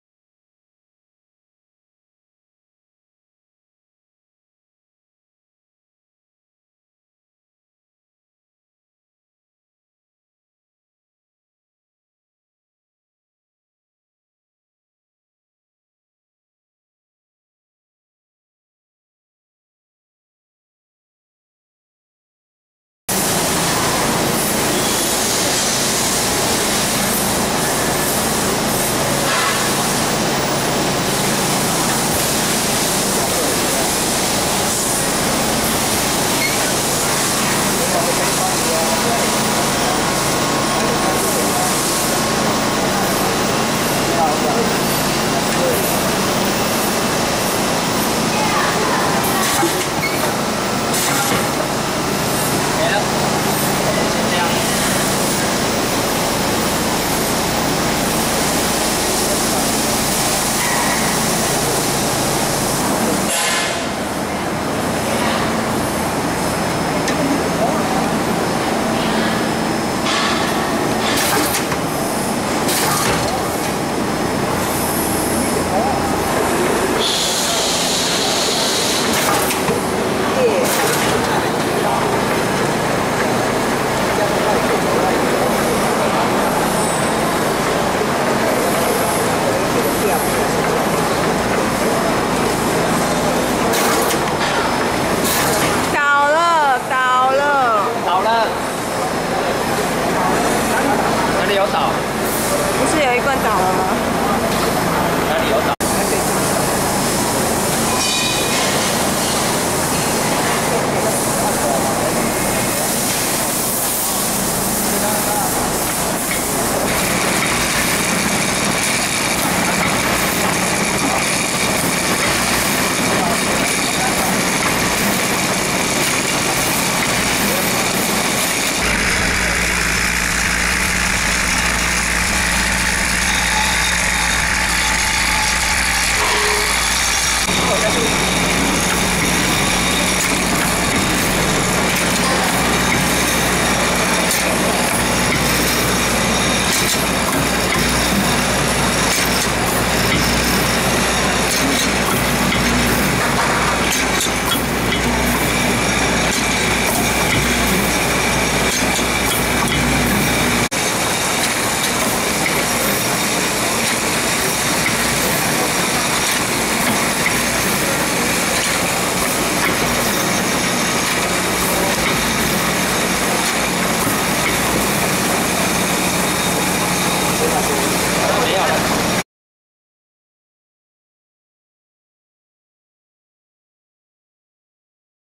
益芳封口機(YF SUPER SEALER)ET- R8全自動封口機 罐子 Sealing Machine(Jar)
益芳封口機(YF SUPER SEALER)
1.型號：ET-R8
2.模數：上模x8+下模夾具
3.封口容器：罐子
4.功能：輸送帶進杯→圓盤進杯→填充→封口→鎖蓋→輸送帶出杯

(A)Model：ET-R8
(B)Cavity：Upper mould for 8 cavities+ Container Clampx 1Set
(C)Tray Type：(Jar)
(D)Function： Conveyor-In →Capper→ Liquid filler→Sealing→Cappering→Conveyor-Out